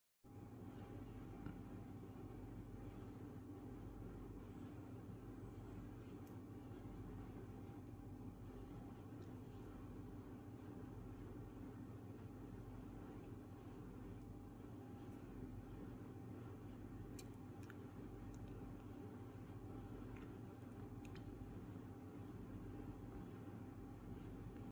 Batu Labradorite Star Of David Hexagram Super No Hole 27mm G275
100% Asli Batu Labradorite Star Of David Hexagram Super No Hole Full Natural Stone

1. menambah d*ya tar*k
2. tena*gkan pikiran
3. menambah keperc*ya diri
4. mempertaj*mkan inst*nct
5. netralisir en*rgy neg*tive
6. menambah spir*tual pow*r

asal: India
ukuran: 27 x 27 x 10mm
berat: 13.7g
kode: G275